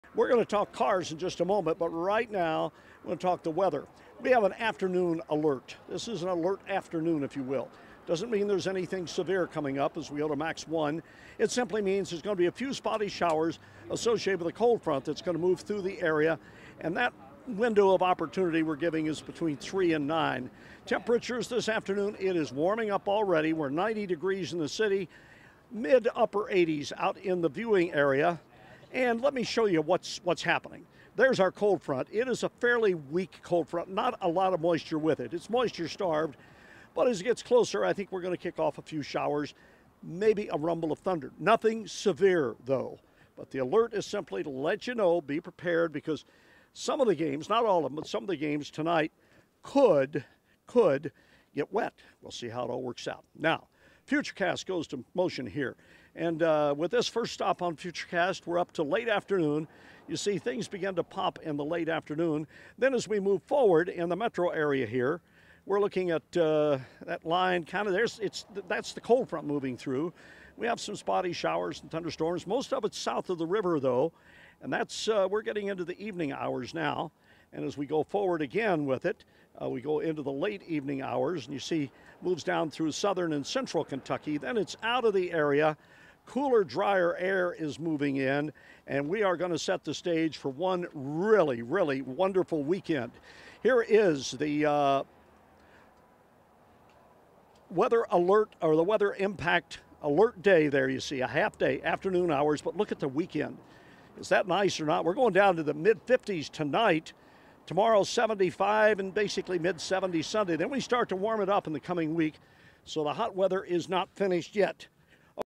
Hot afternoon and evening rain | Sept. 6, 2024 WHAS11 noon weather
90° heat with some afternoon thundershowers. We will dry out & cool down big time tonight! Low temperatures in the 50s & high temperatures in the 70s this weekend.